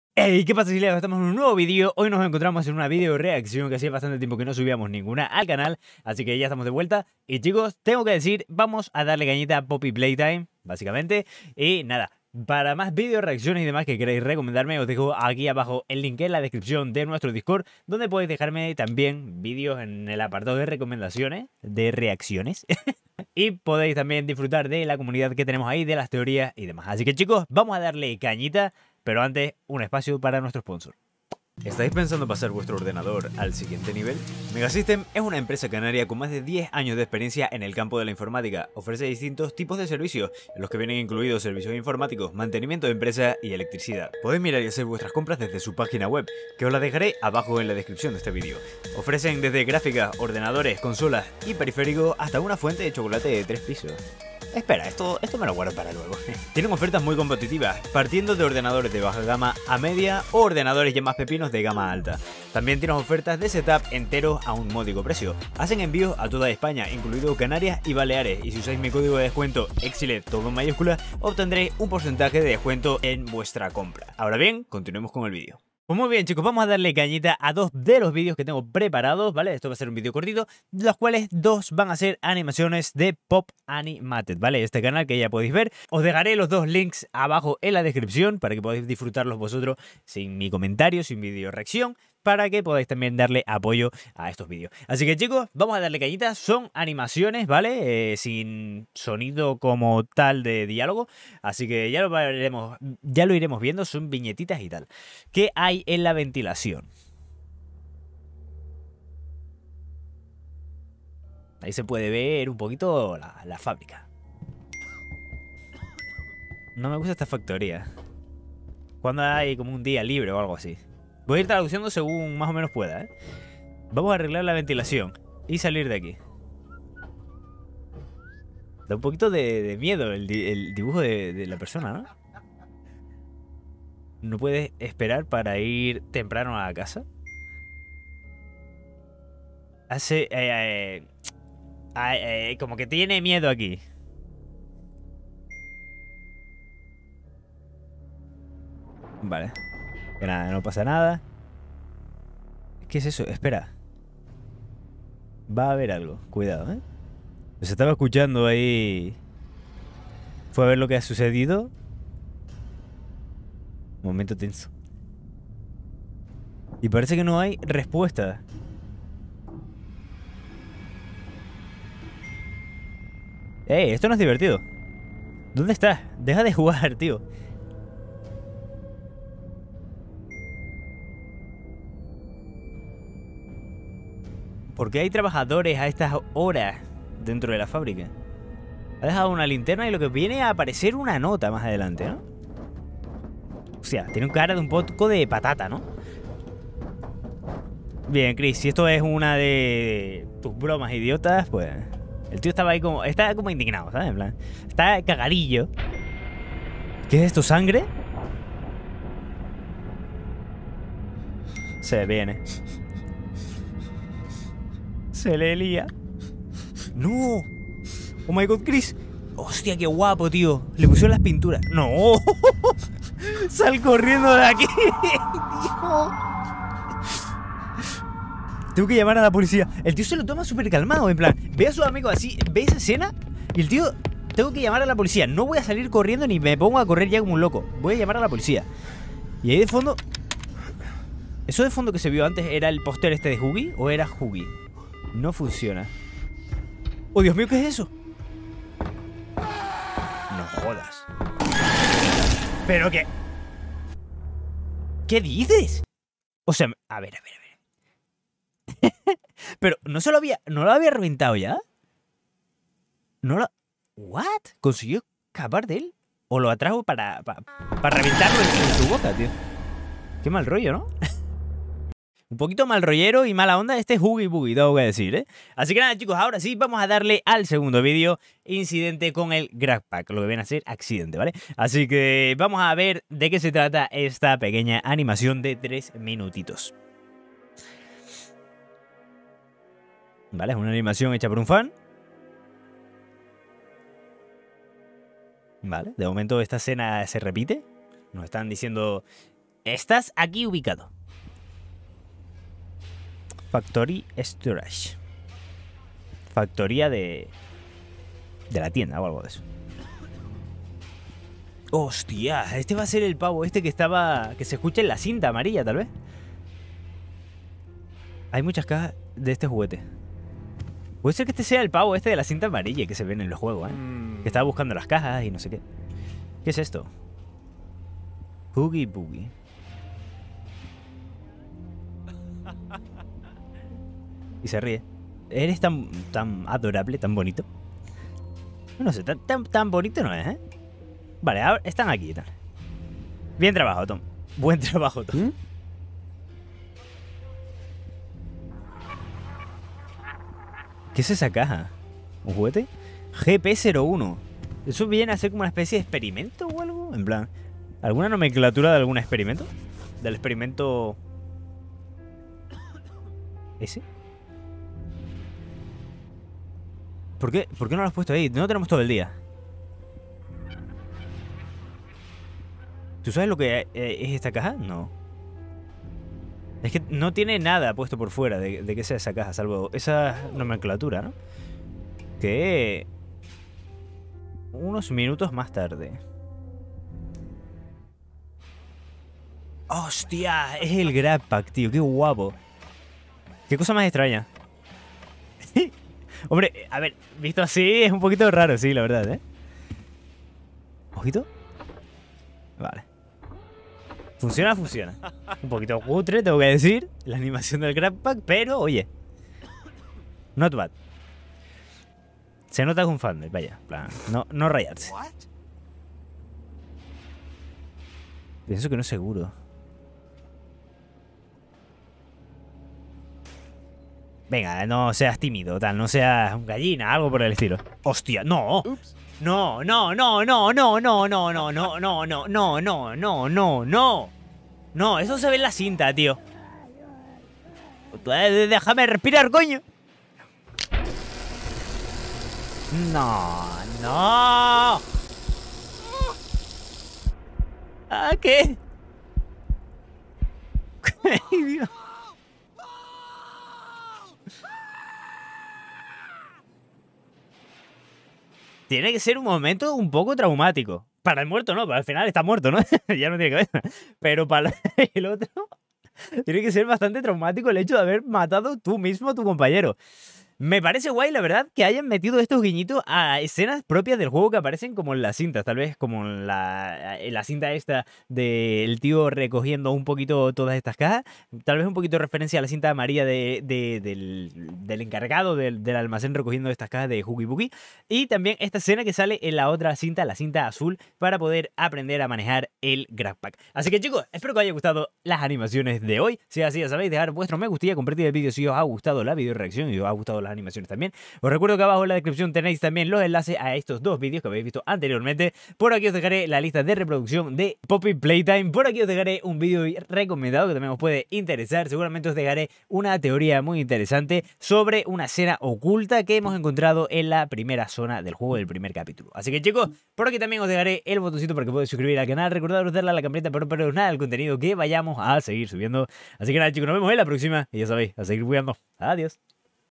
HUGGY WUGGY SIEMPRE TE OBSERVA 😨 REACCIONANDO A ANIMACIONES DE POPPY PLAYTIME Español
✨ Aconsejo ver el vídeo a oscuras y con auriculares ✨
QUE PASA EXILIADOS!!! Hoy os traigo una nueva video reacción del aclamado Poppy Playtime. En esta ocasión reaccionaremos a 2 videos coritos de un estilo de animación que no habíamos visto, basando su dialogo en viñetas. Acompañadme a mirar las aventuras que nos esperan en Playtime Co. 🤪
Espero que os guste el vídeo, si es así dadle amor al vídeo para yo saber que os ha gustado ;3
Recordad dejar vuestro comentario y compartir el video 💙

EXILED